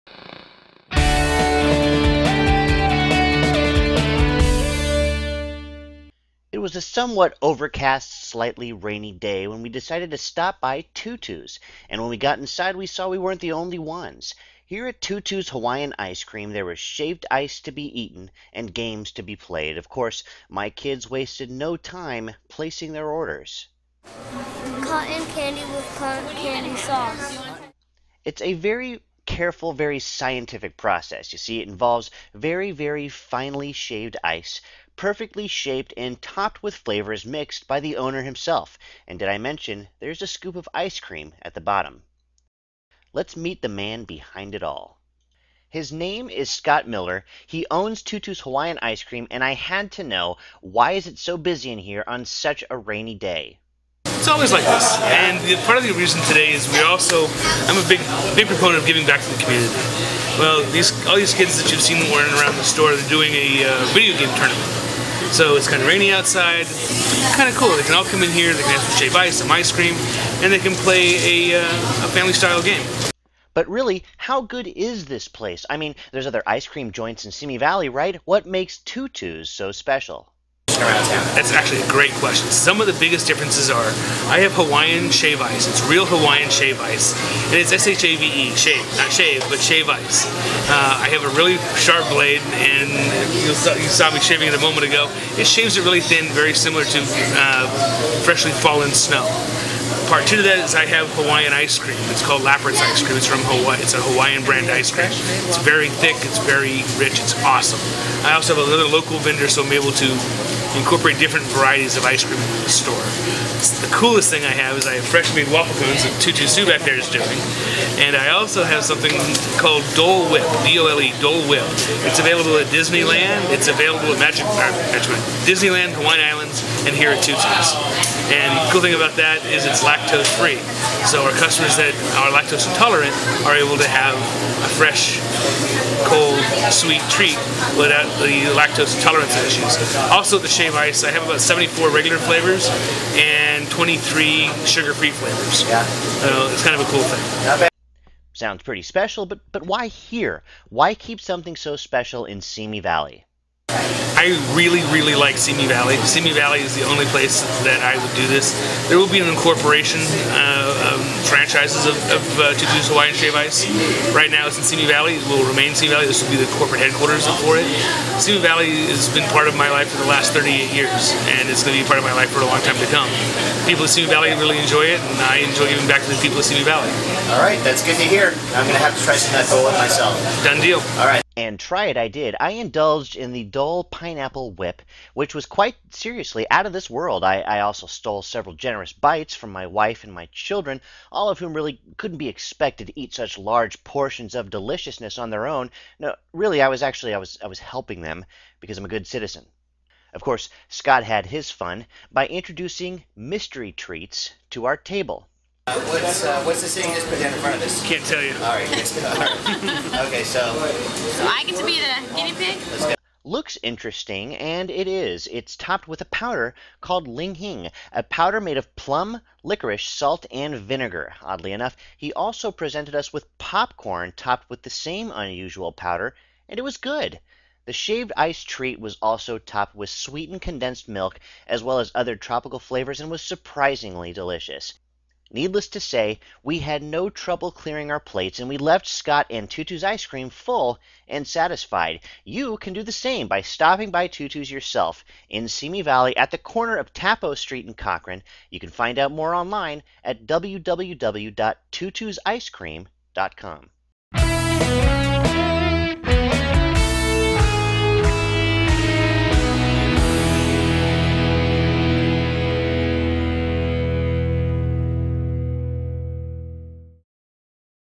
Tutu's Hawaiian Ice Cream Shack
I had heard a lot of good things about this place, but I had no idea how good it would end up being.  If you haven't taken your friends and family to Tutus Hawaiian Ice Cream Shack in Simi Valley, you're missing out.  Get there immediately.  Watch the video and enjoy!